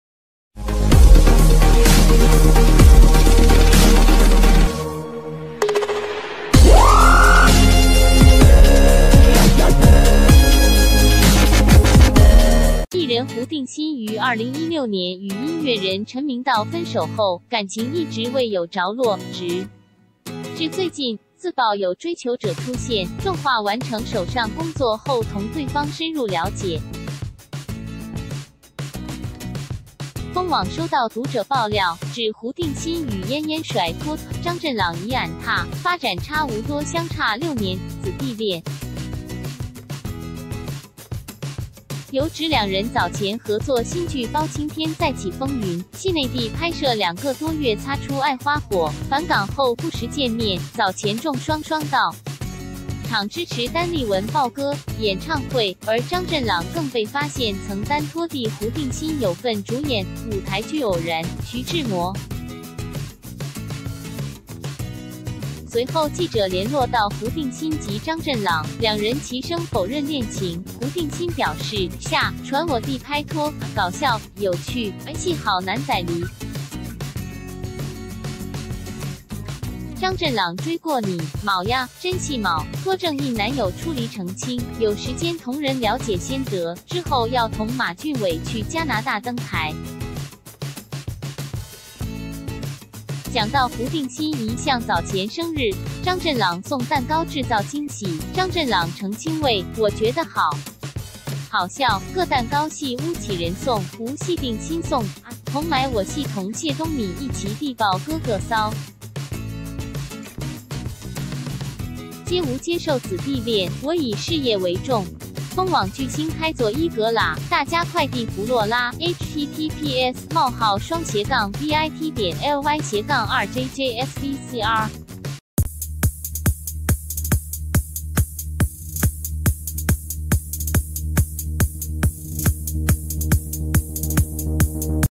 被爆暗撻張振朗！胡定欣：佢係好男仔
藝人胡定欣於2016年與音樂人陳明道分手後，感情一直未有着落，直至最近佢自爆有追求者出現，仲話完成手上工作後同對方深入了解。東網收到讀者爆料，指胡定欣與啱啱甩拖嘅張振朗疑暗撻，發展差唔多相差6年嘅姊弟...